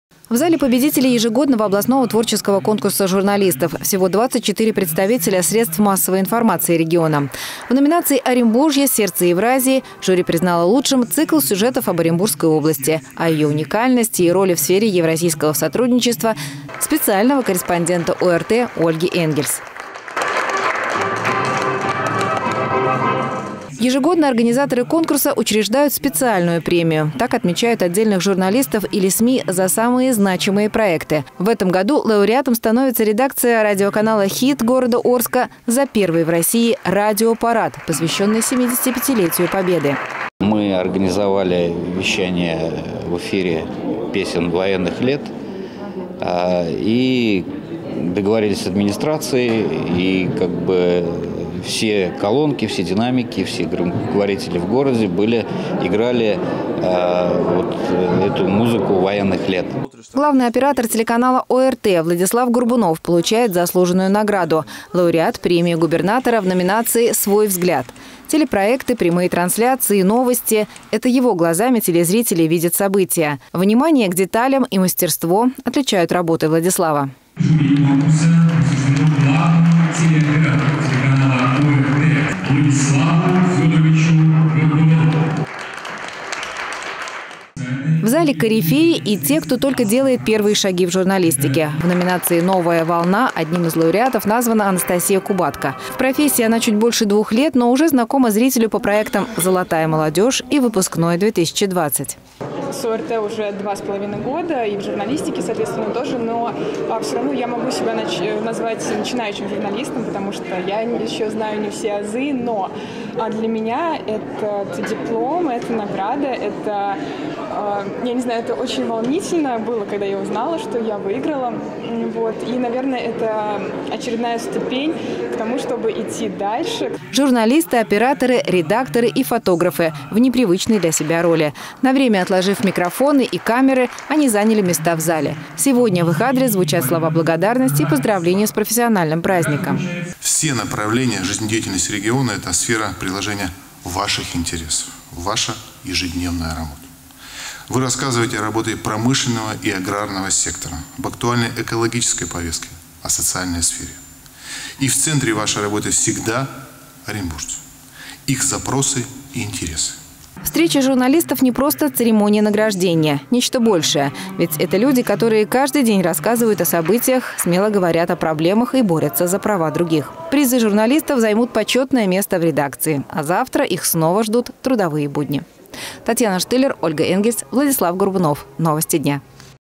В ДЕНЬ ПЕЧАТИ
Поздравления и награды получают представители не только печатных СМИ.  К этому дню в правительстве области приурочили торжественную церемонию вручения премий по итогам ежегодного творческого конкурса среди сотрудников и коллективов средств массовой информации и полиграфических изданий за 2020 год.